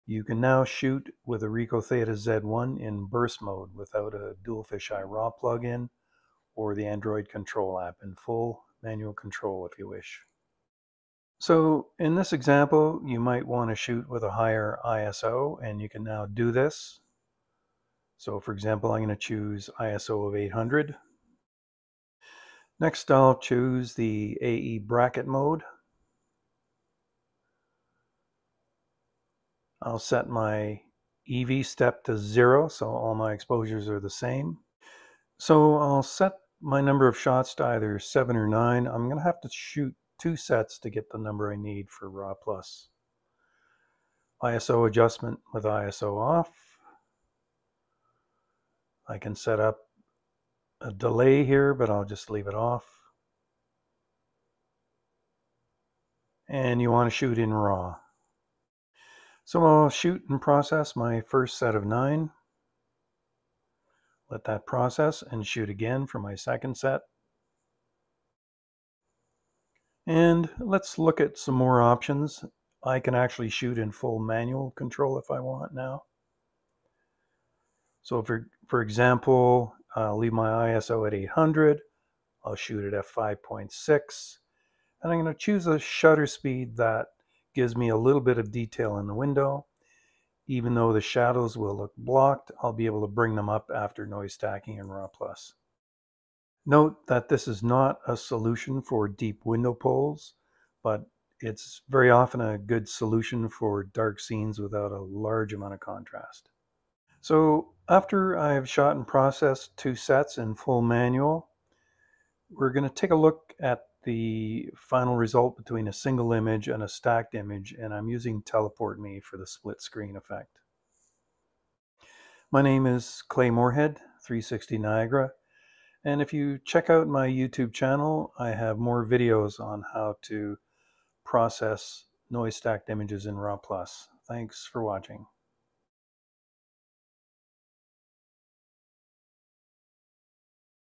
New AEB EV 0 option for Theta Z1
You can now shoot a series of similar exposures without the need for an additional plugin or control app. These images can be processed in Raw+ for a noise free image.